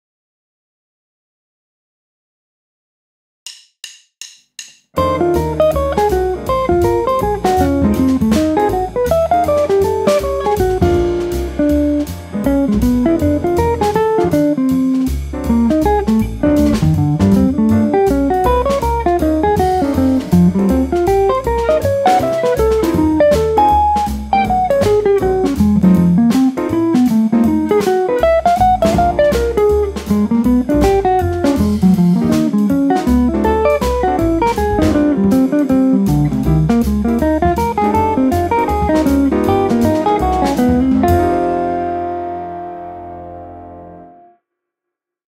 Quartal Blues - F Blues
Jazz improvisation on F Blues connecting triads of fourths by Stefano Micarelli